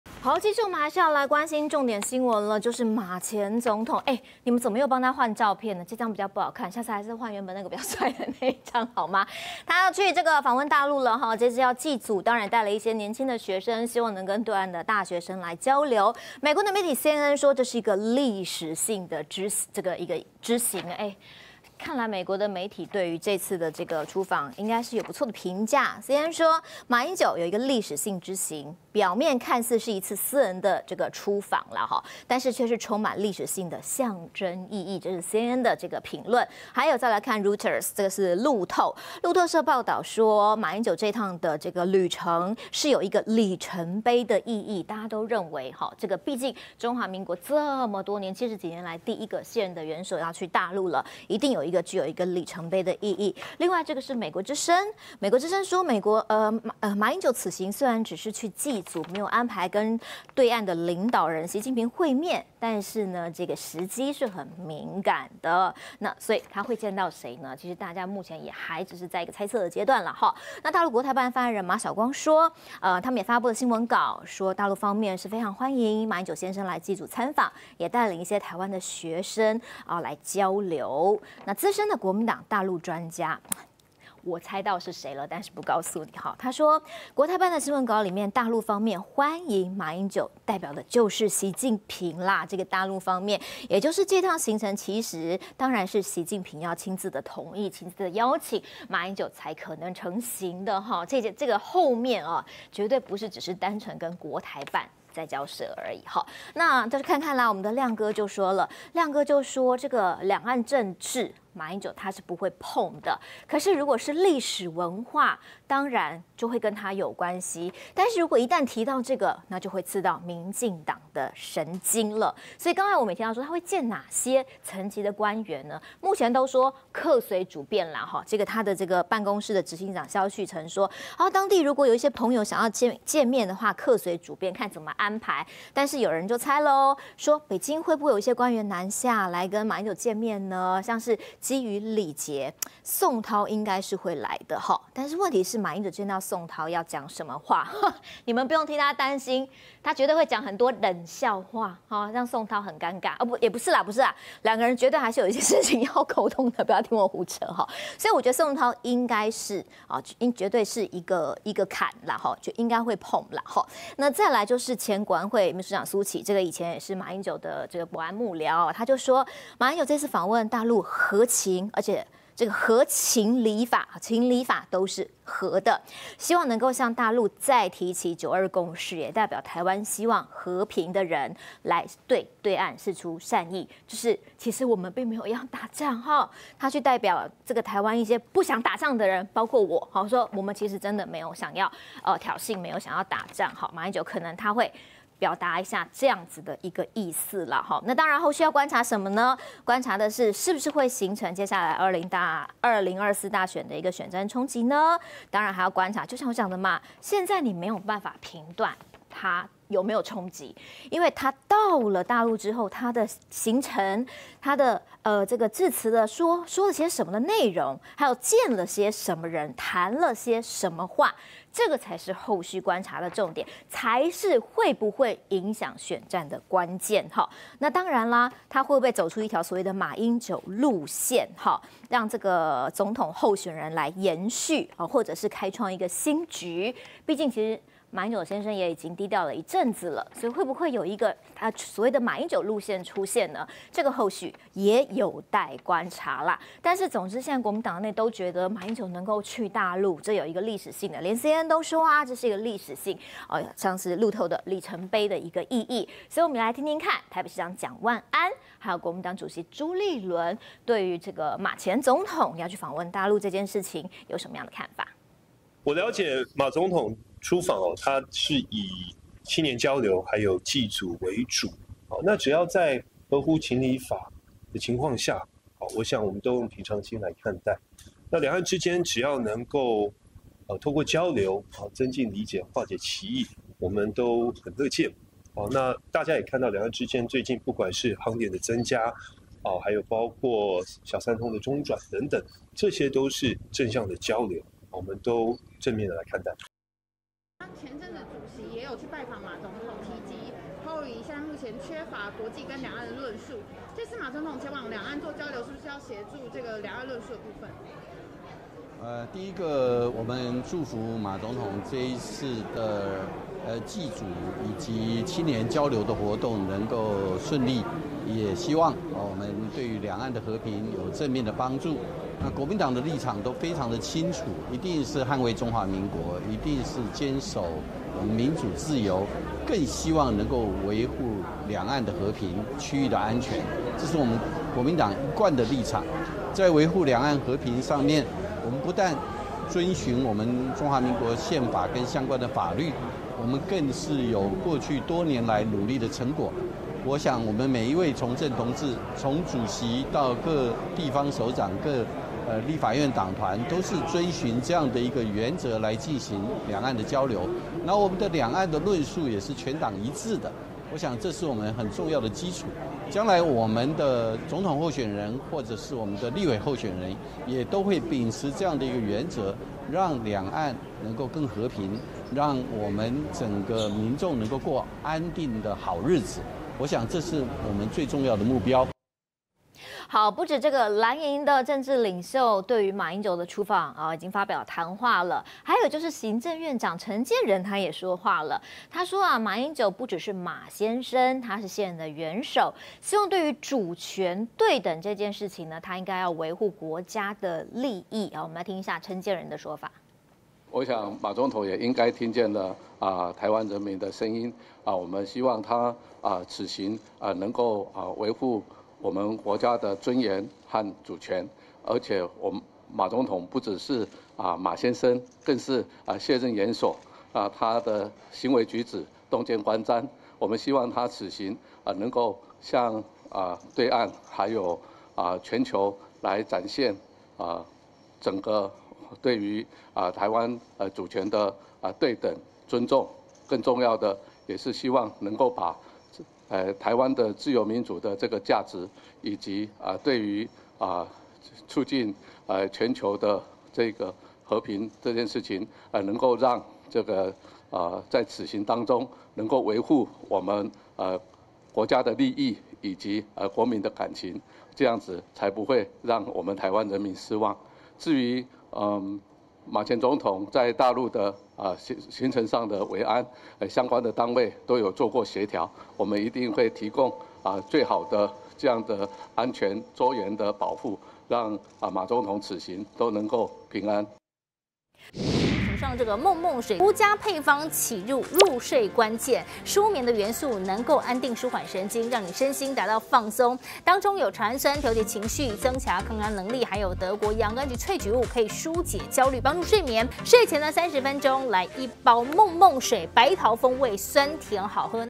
【每日必看】馬英九訪中有助兩岸論述 朱立倫:全黨立場一致｜馬英九赴陸祭祖!挺兩岸交流 蔣萬安:合乎情理法@中天新聞CtiNews 20230321
00:00 大陸行"馬先生"客隨主便 國台辦歡迎 總統府尊重
01:27 蘇起盼馬提九二共識 學者:交流無須過度政治想像
01:47 若馬英九赴陸談"這事" 郭正亮驚爆:會刺到民進黨神經
02:23 馬辦強調兩岸一定要交流 一中各表成馬登陸考驗
04:26 馬英九赴陸祭祖!挺兩岸交流 蔣萬安:合乎情理法
05:00 馬英九訪中有助兩岸論述 朱立倫:全黨立場一致
06:20 馬赴陸祭祖 陳建仁盼"主權對等":維護國家利益